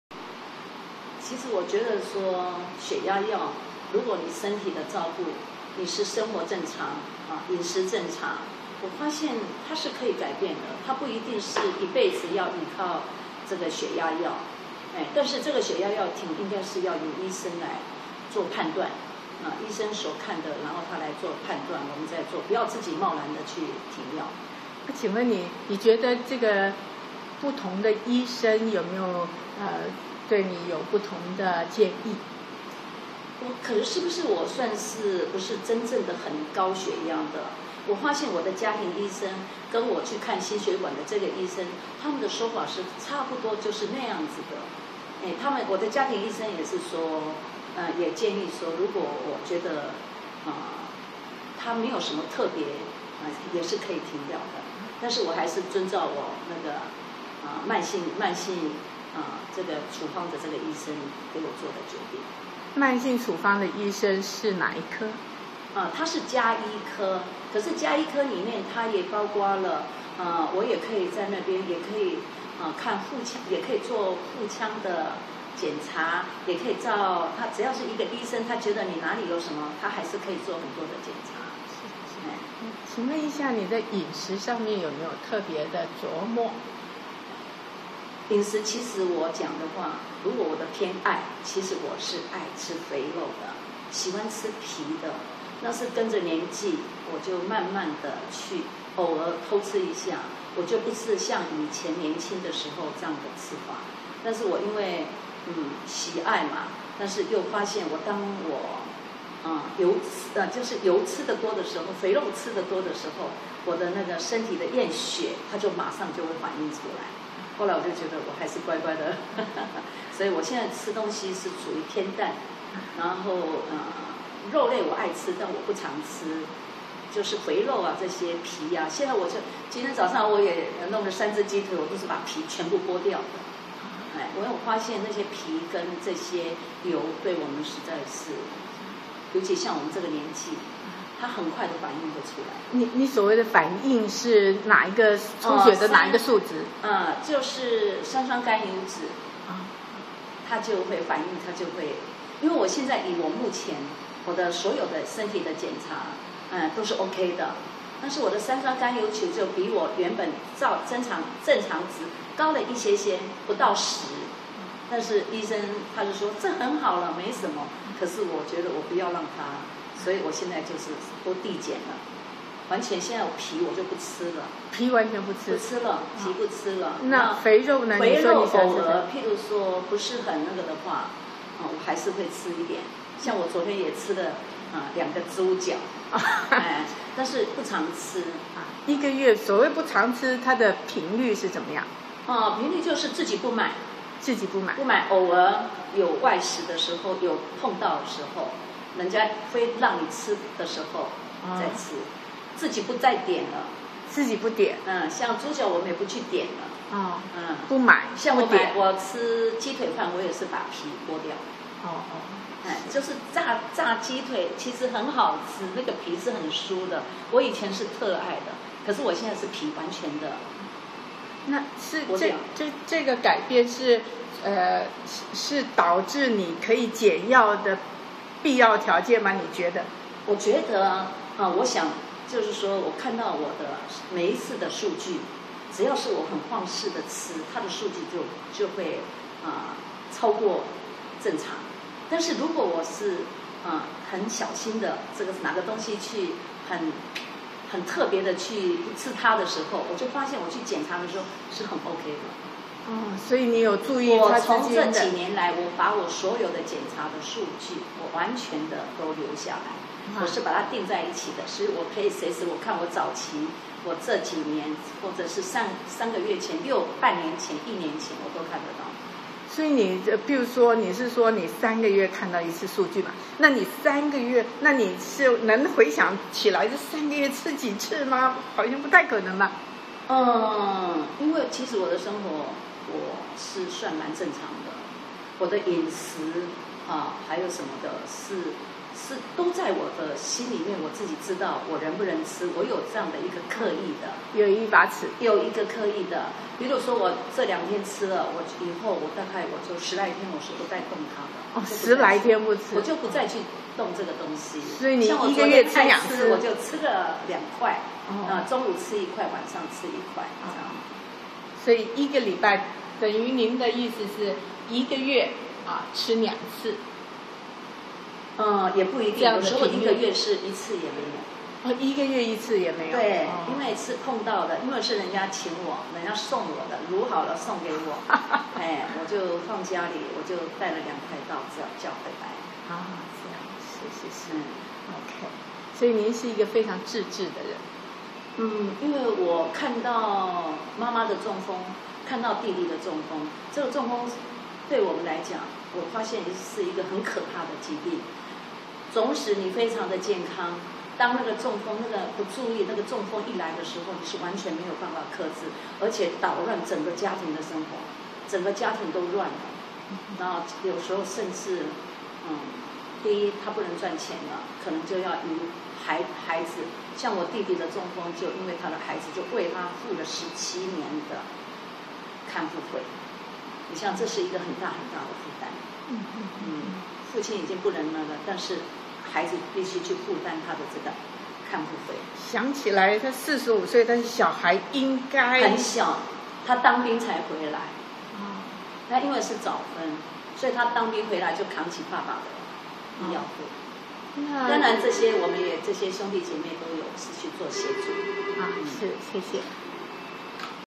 聽從醫生對高血壓藥停藥的建議成功停藥，飲食方面節制對高血壓有絕對因素VIDEO0351
成功戰勝家族遺傳高血壓陳敏華接受基督得著平安